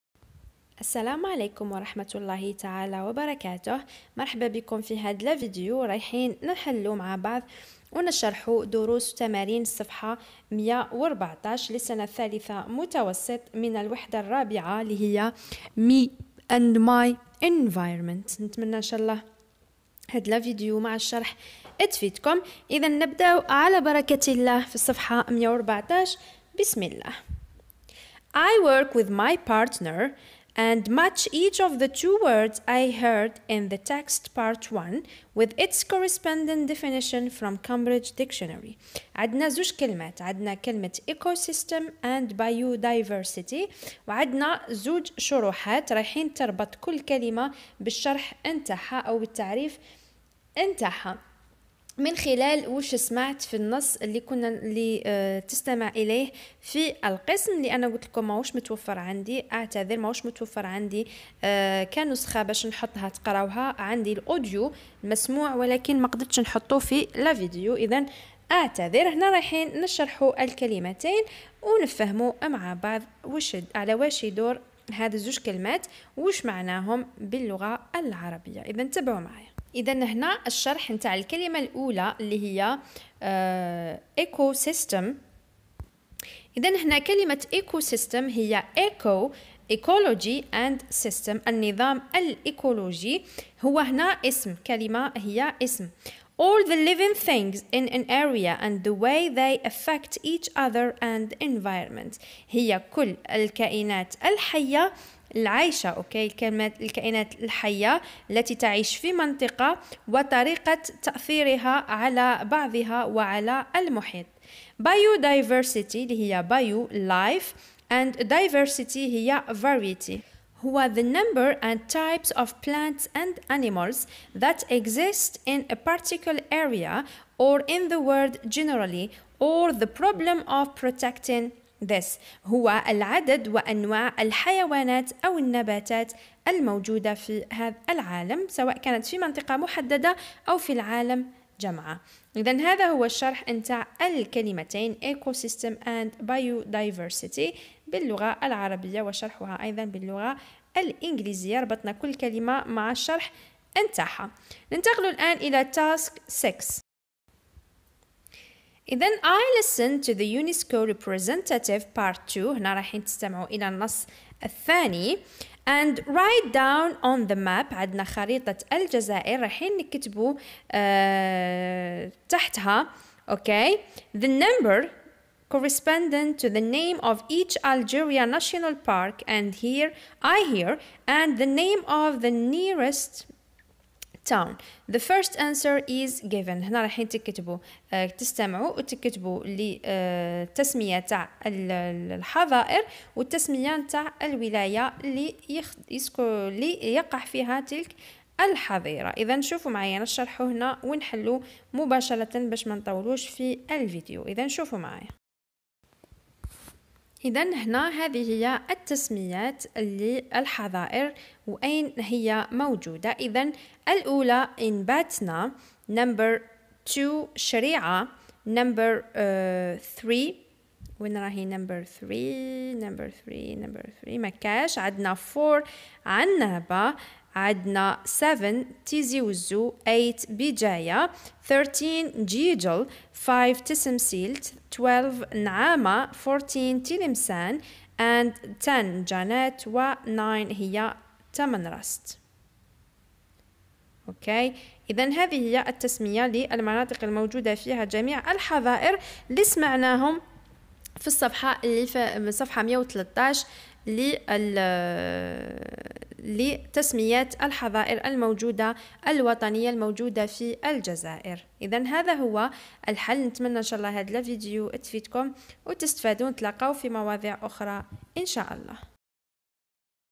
شرح دروس وحلول تمارين الصفحة 114لتلاميذ السنة الثالثة متوسط الوحدة الرابعة
شرح دروس الكتاب المدرسي لغة انجليزية لتلاميذ السنة الثالثة متوسط لغة انجليزية
حلول تمارين الكتاب المدرسي لغة انجليزية لتلاميذ السنة الثالثة متوسط لغة انجليزية